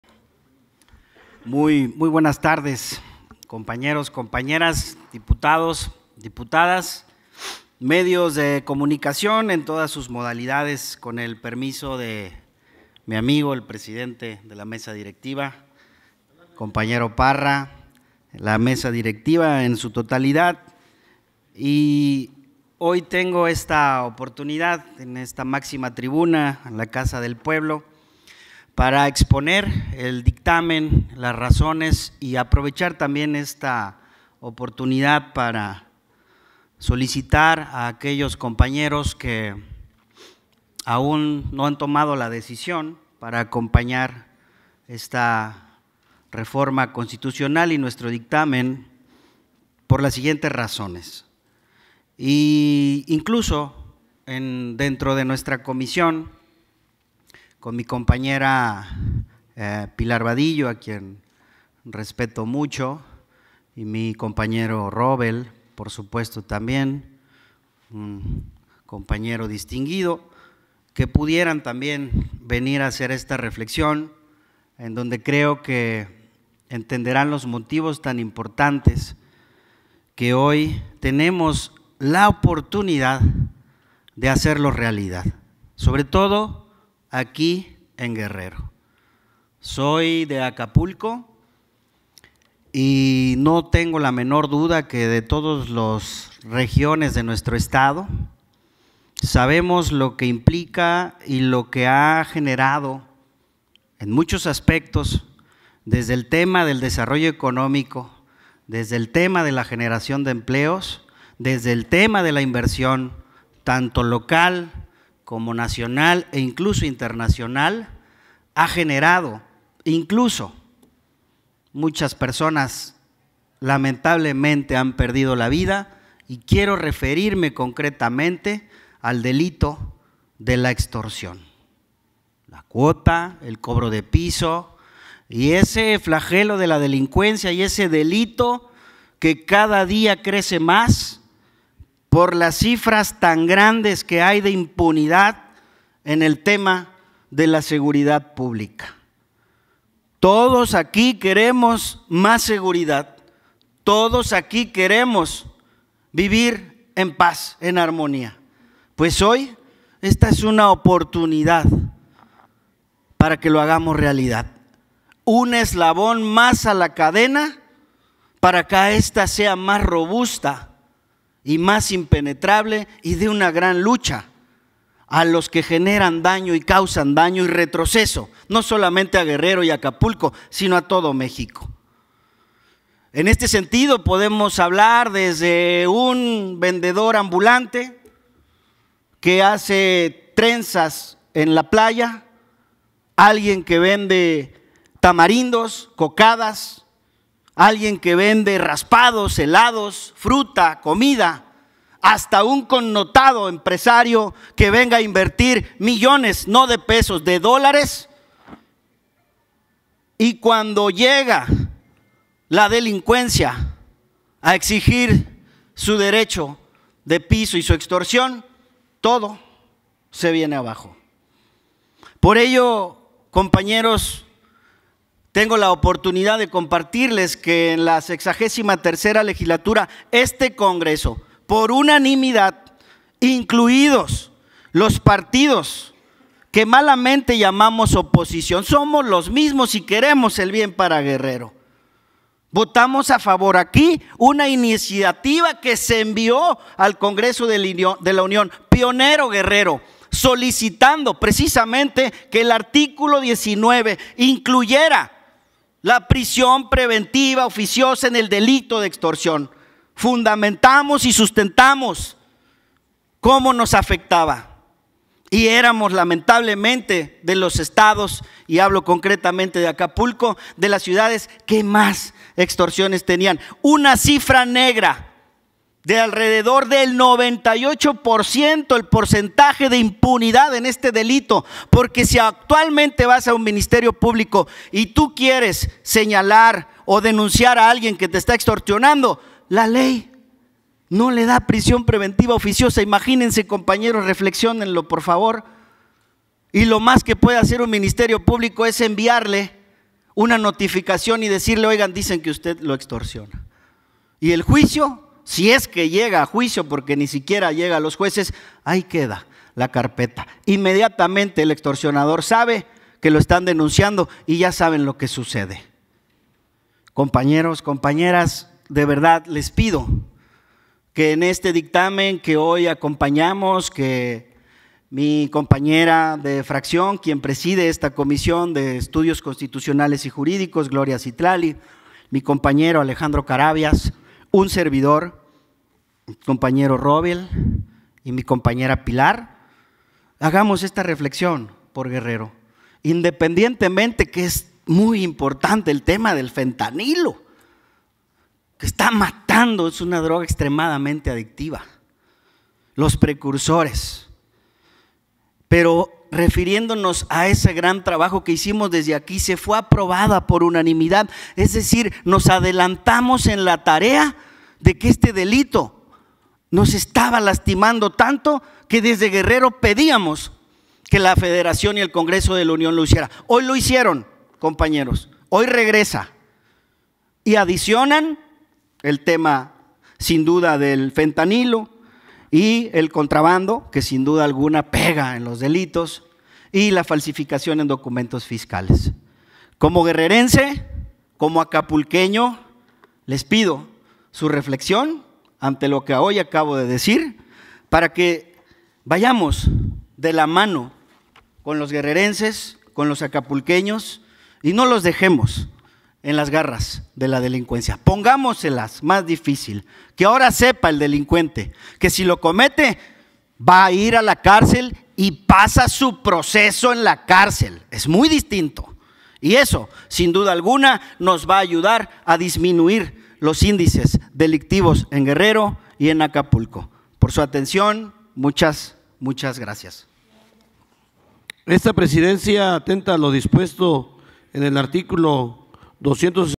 Joaquín Jacko Badillo Escamilla presenta dictamen sobre reforma a la prisión preventiva oficiosa
El Pleno del Congreso del Estado aprobó un dictamen relativo a la reforma al artículo 19 de la Constitución Política de los Estados Unidos Mexicanos en materia de prisión preventiva oficiosa, turnado por el Senado de la República. El diputado Joaquín Jacko Badillo Escamilla, integrante de la Comisión de Estudios Constitucionales y Jurídicos, fundamentó la importancia de esta reforma en la lucha contra la inseguridad, destacando que es un eslabón más para brindar justicia a las y los guerrerenses.

Mira el video completo aquí.